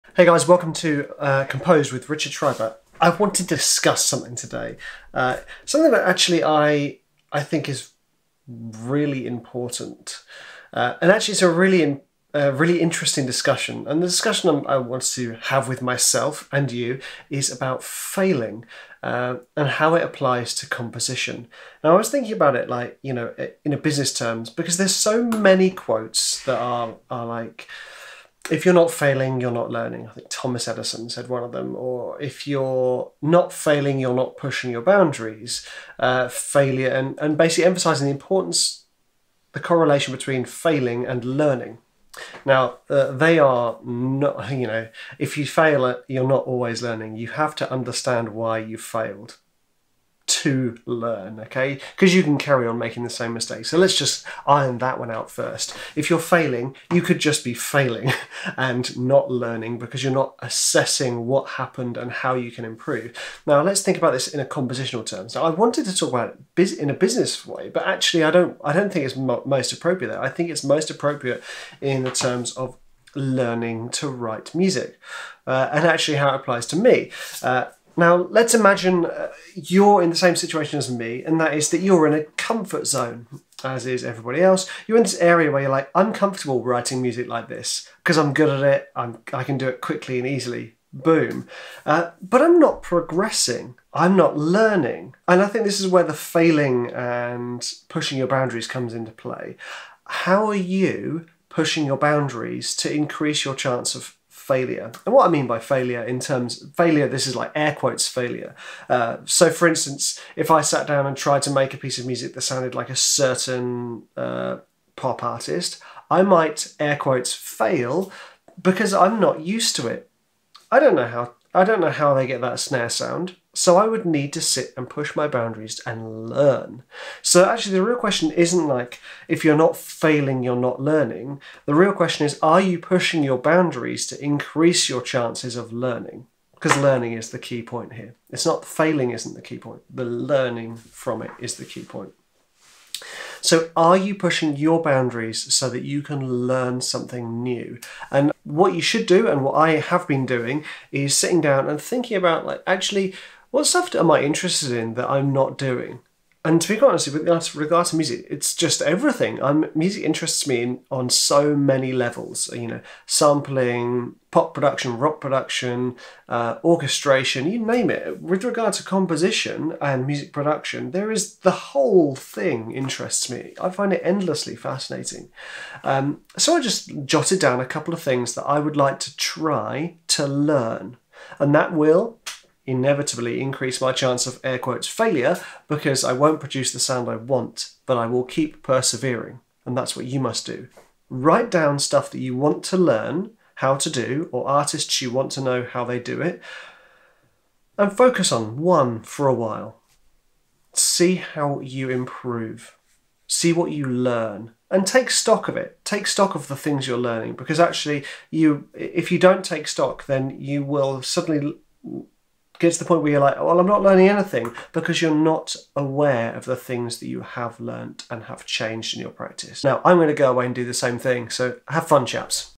Are You Learning From Your Mistakes?
It's important to think about why we fail, so we can learn from it!